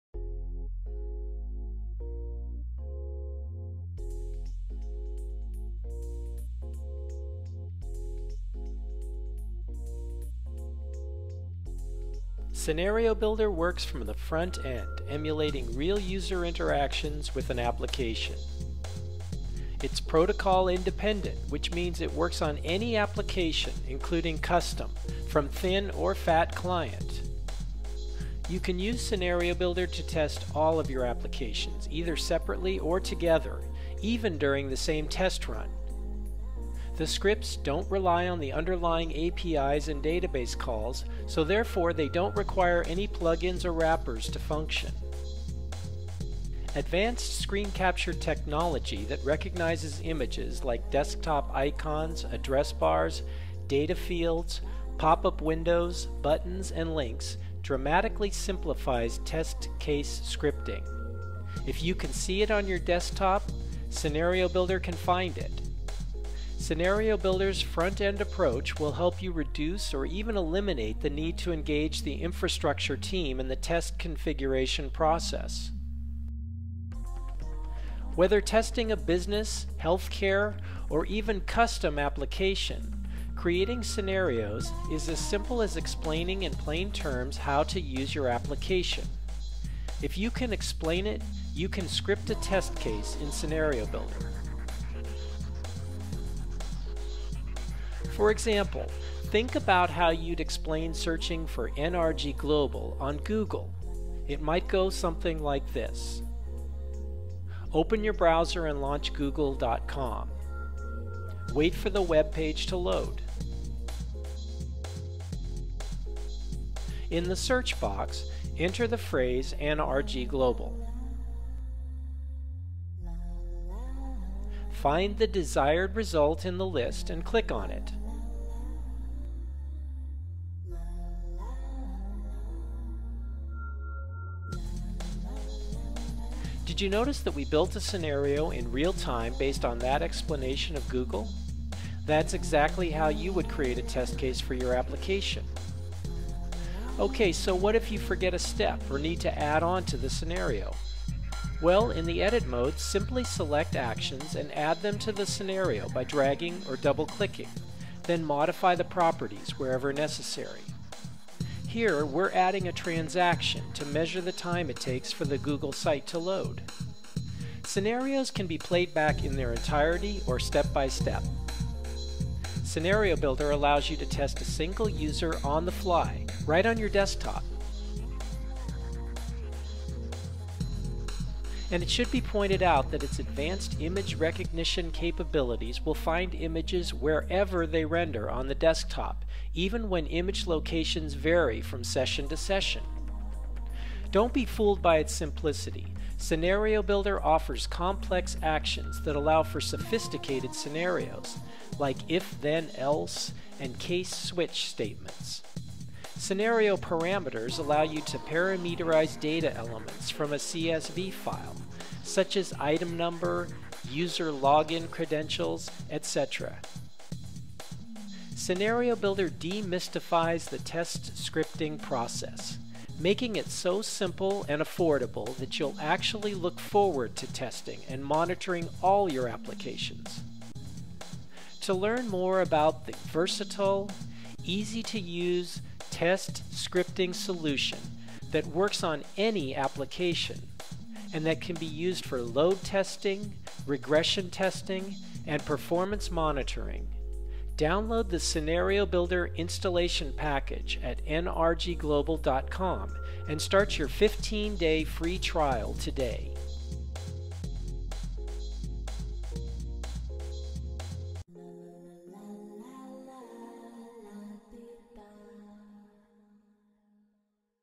ScenarioBuilder - Easy-to-Use Test Scripting Solution by NRG Global
ScenarioBuilder is the versatile test scripting tool that works on any application accessible from a Windows front end.  Watch this short video to learn how this amazing tool can simplify the Load Testing, Regression Testing and Performance Monitoring of all your applications!